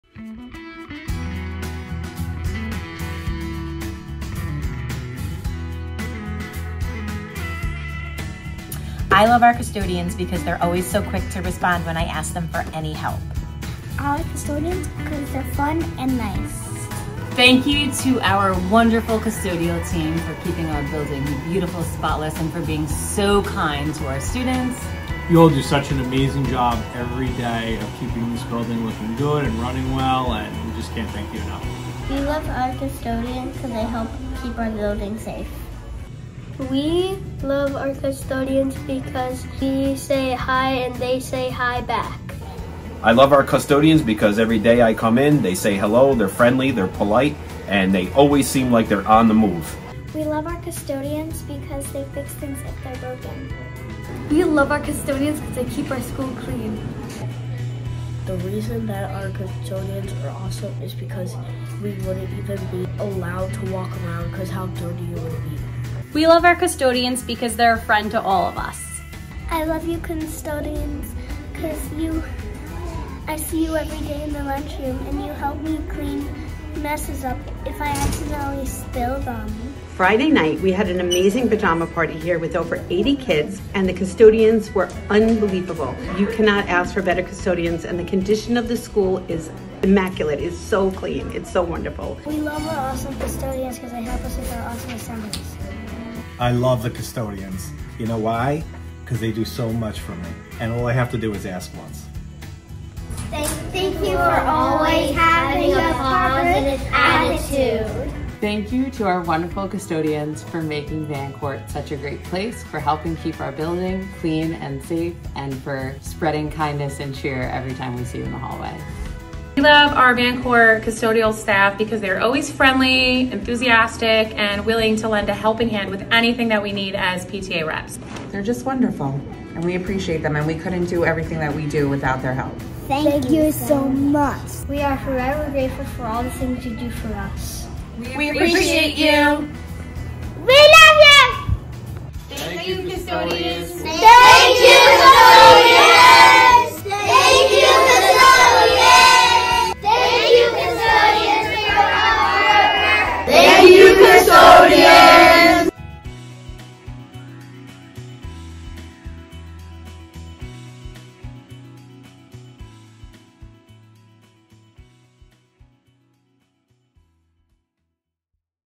Custodian Appreciation Day 2023
Our students came together to share their appreciation for our wonderful custodians!